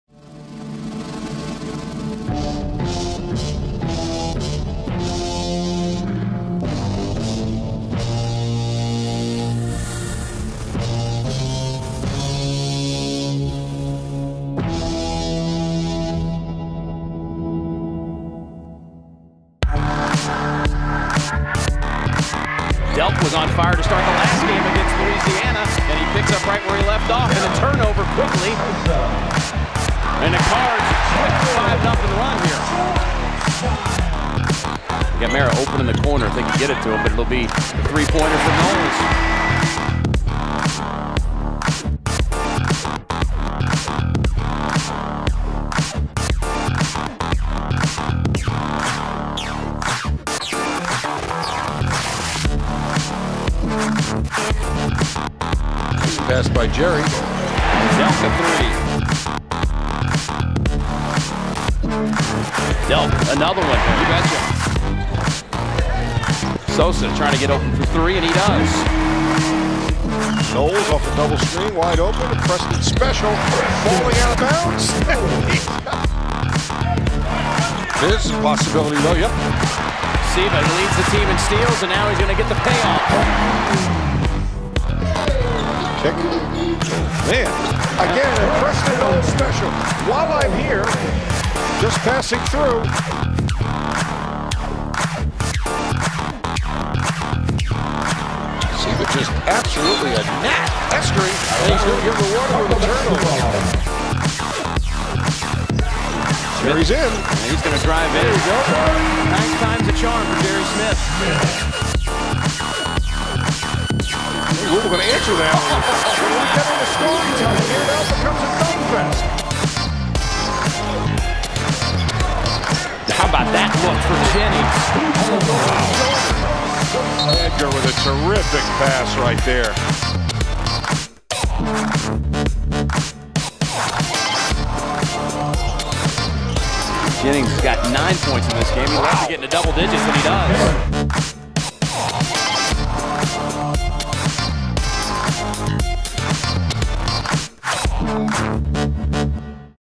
Louisville vs. Radford 12-27-09
College basketball highlights from Louisville vs. Radford on December 27, 2009